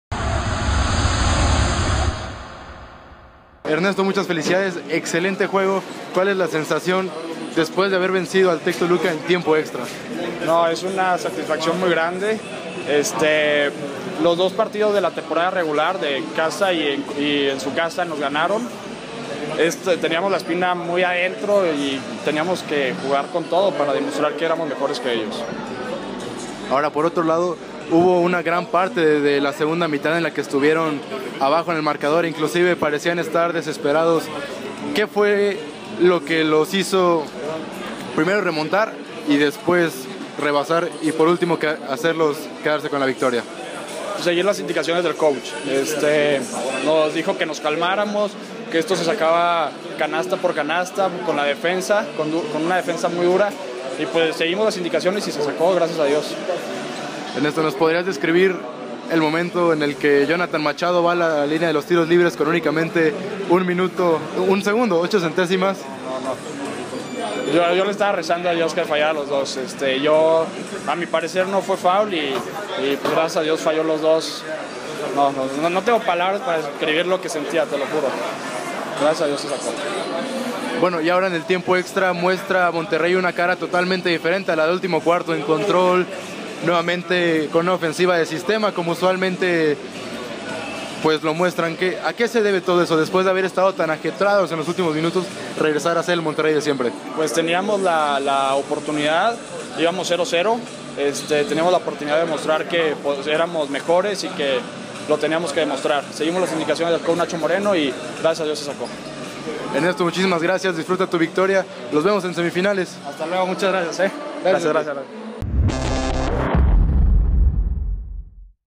Entrevista con Ernesto Morales, jugador del ITESM MTY.
Los 8 mejores equipos de la liga inician su camino en la búsqueda del tan ansiado título de la Liga de la Asociación de Basquetbol Estudiantil.
TVUDLAP transmite todos los partidos del baloncesto universitario de los Ocho Grandes